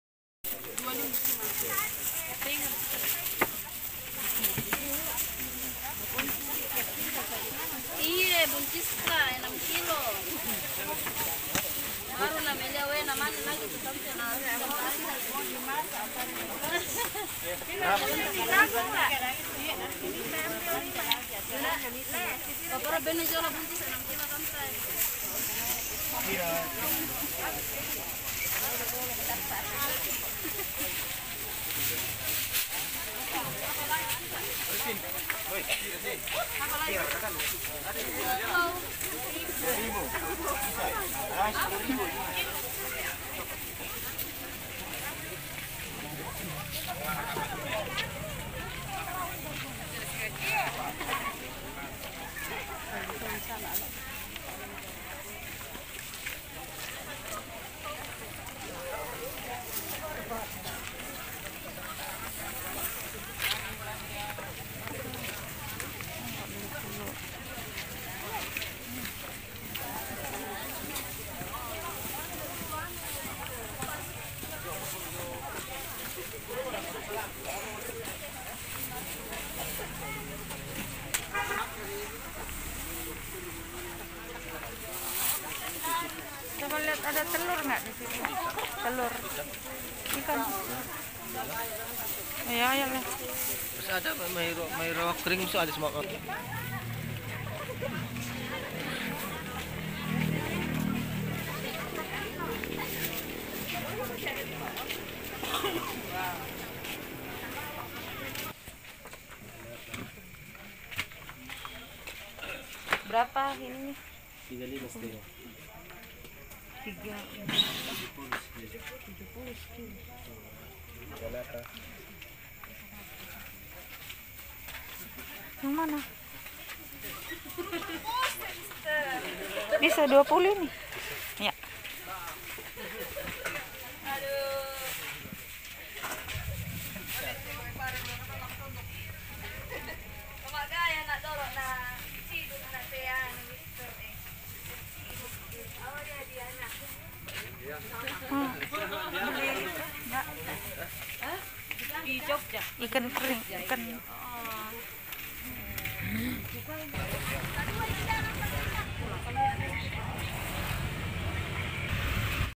Aktivitas Pasar Subuh Yang Ramai Di Serbu Pedagang, Rantepao Toraja Utara
pasar subuh adalah salah satu pasar yang ada di Toraja tepatnya di jalan poros Rantepao Makale, pasar ini adalah pasar pemasok sayur-sayuran yang datang dari sudu, Enrekang dan sekitarnya, kemudian masyarakat Toraja membeli untuk di jual knali baik di warung maupun di bawa keliling dengan menggunakan motor.